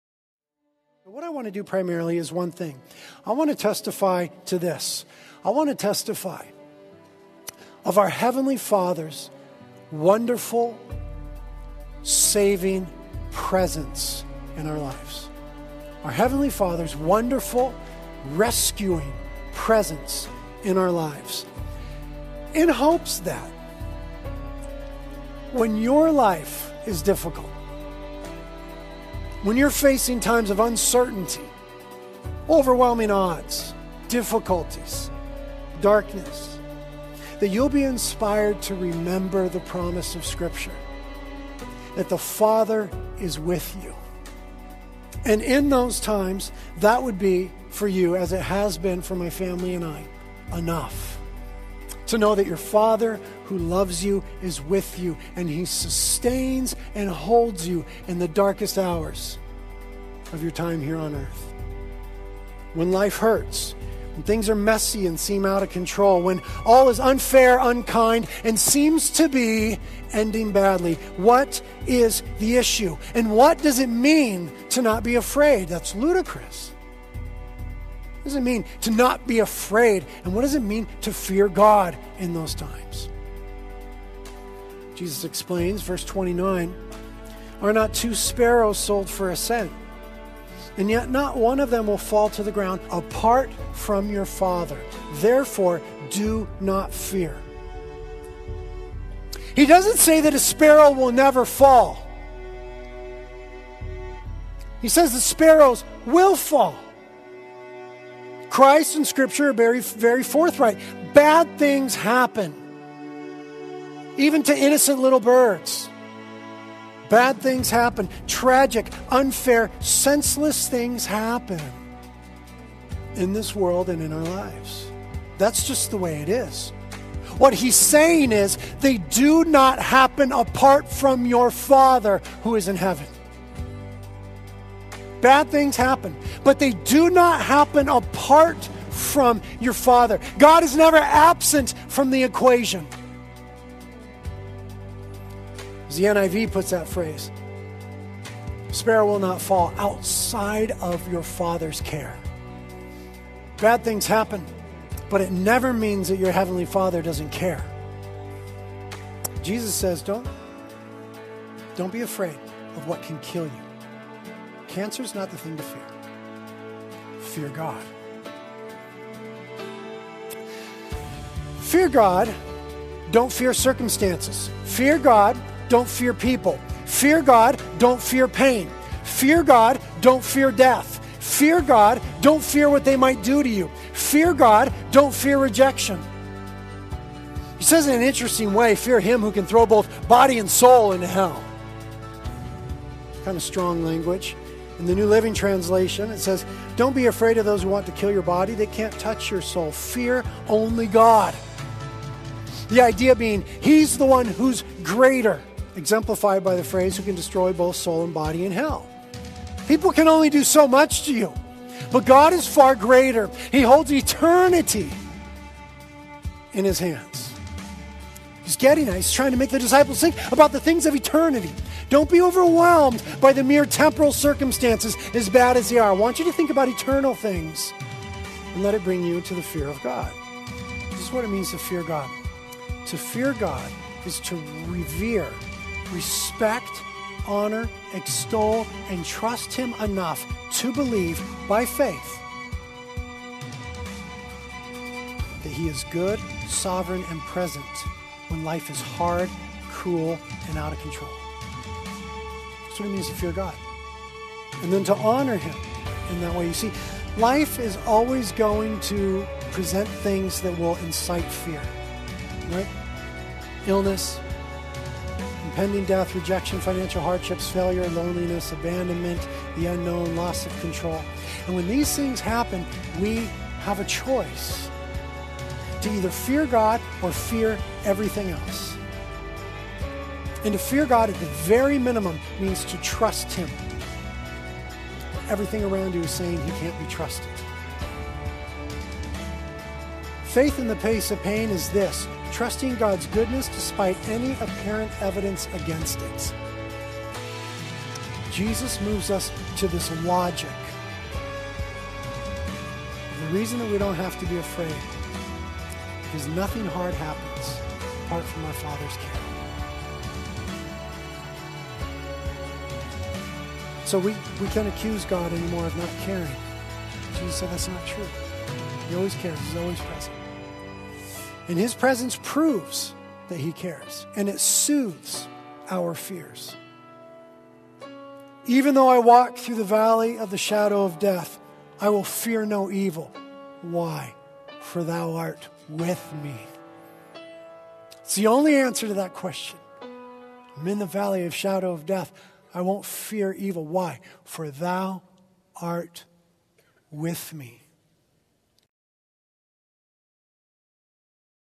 When Sparrows Fall - Britt Merrick Sermon Jam.
" Are not two sparrows sold for a penny? Yet not one of them will fall to the ground outside your Father's care. And even the very hairs of your head are all numbered. So don't be afraid; you are worth more than many sparrows."

~ Matthew 10:29-31.

** Credits to Refresh Hawaii Ministries.